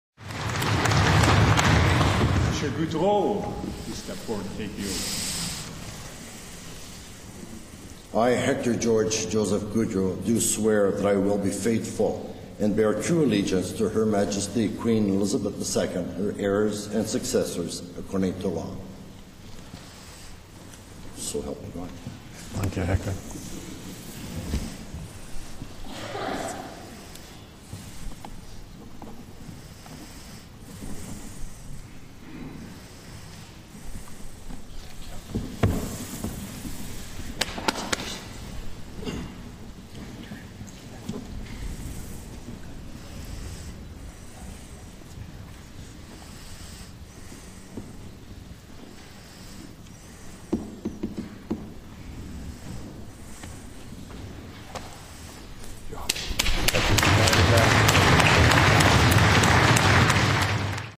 Hector Goudreau is sworn in as MLA for Dunvegan-Central Peace-Notley
Hector Goudreau is sworn in as the returning MLA for Dunvegan-Central Peace-Notley in the Alberta Legislature on May 15, 2012.